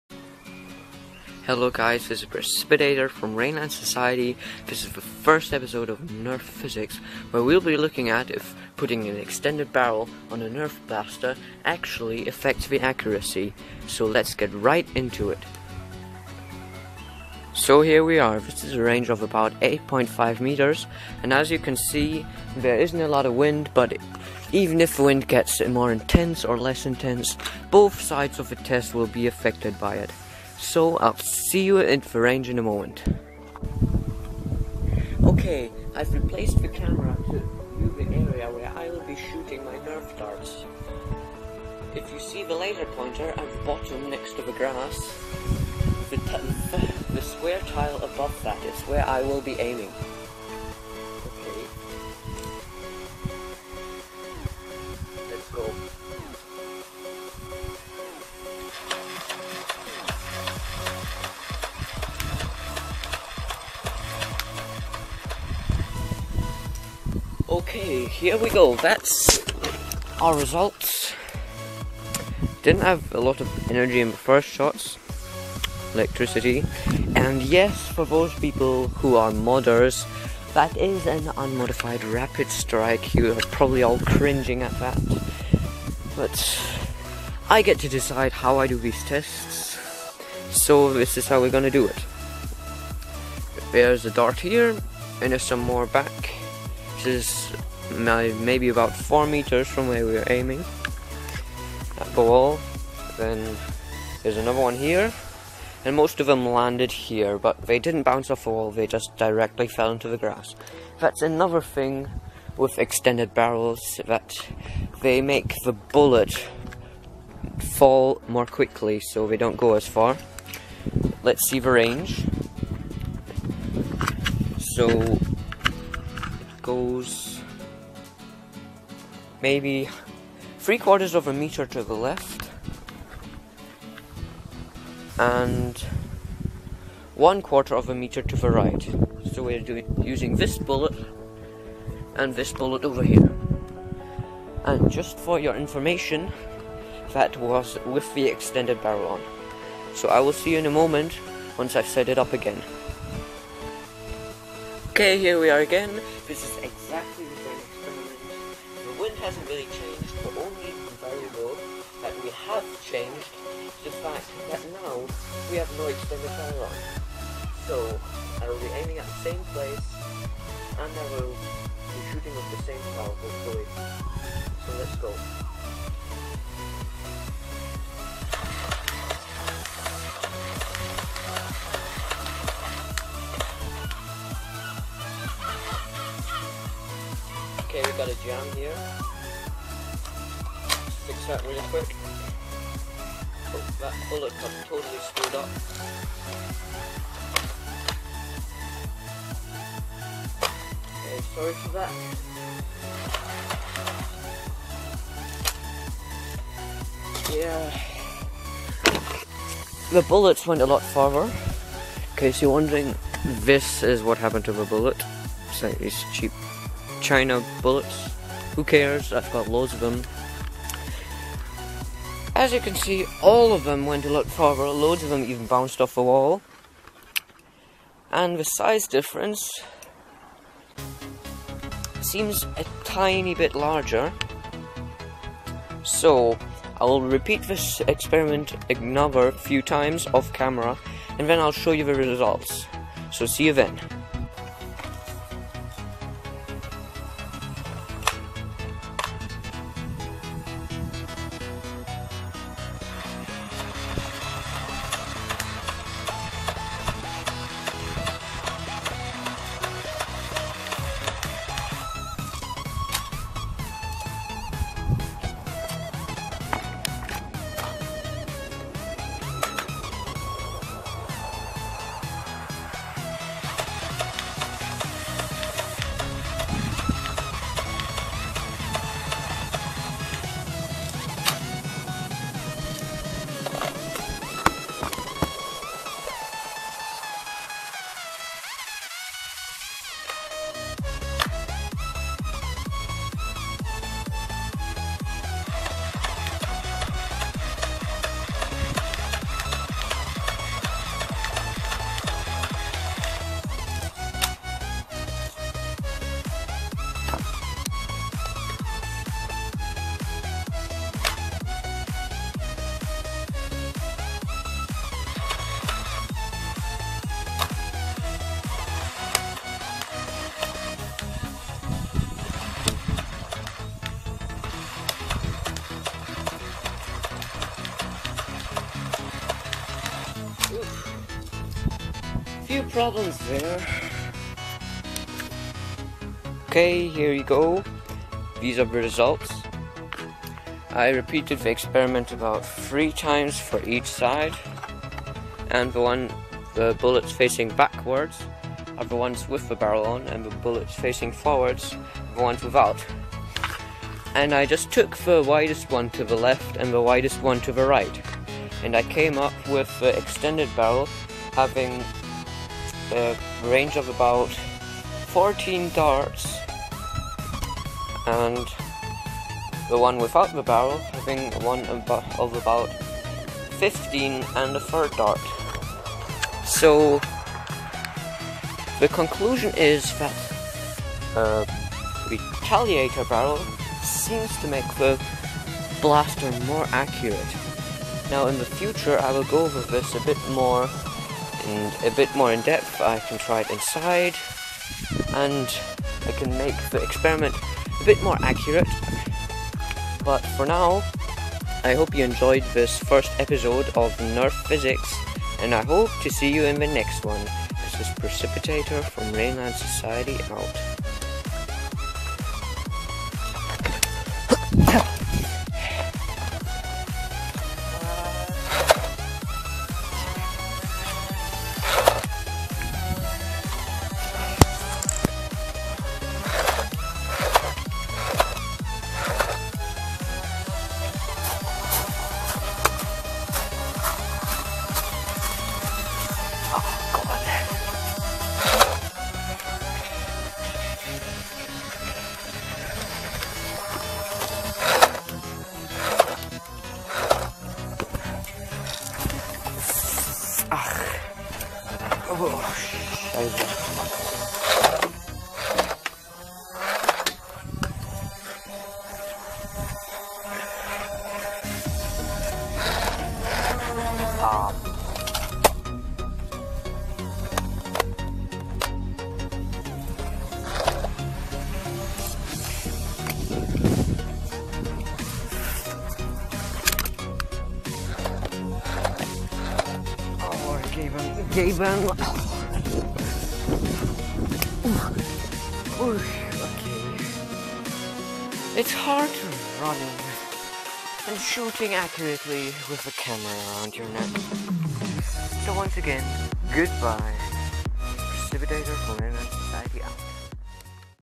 EN | Nerf Physics: #1 - Extended Barrels Part 1
This is part 1 of the Nerf Extended Barrel Accuracy Test.
Sorry for the late upload, I tried to render it months ago but the video editing software kept crashing. Hope you enjoy it. Leave any suggestions in the comments.